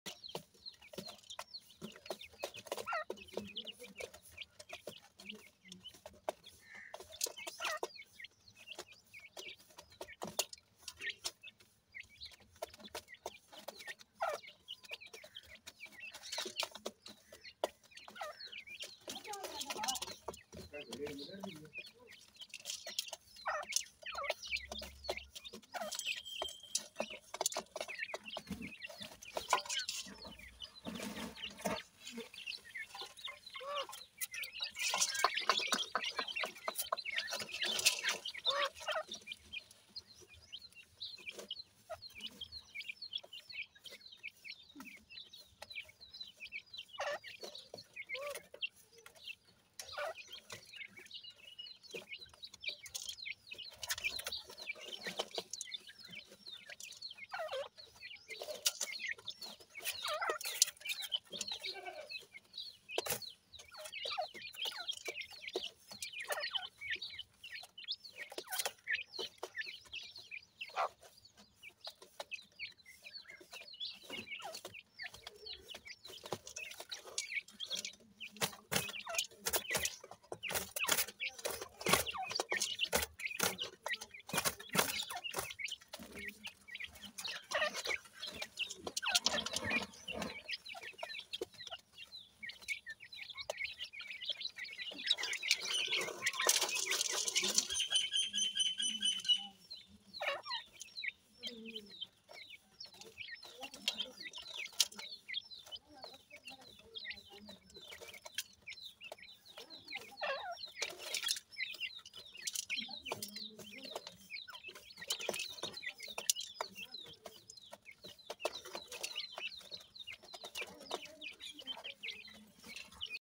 teetar baby khorak,pheasant bird baby, grey francolin chicks
Welcome to our avian consideration corner
In this video, we'll investigate the craft of giving the right sustenance to teetar (partridge) children, fowl bird chicks, and charming dim francolin chicks
 Go along with us as we share bits of knowledge into their exceptional dietary necessities and care tips.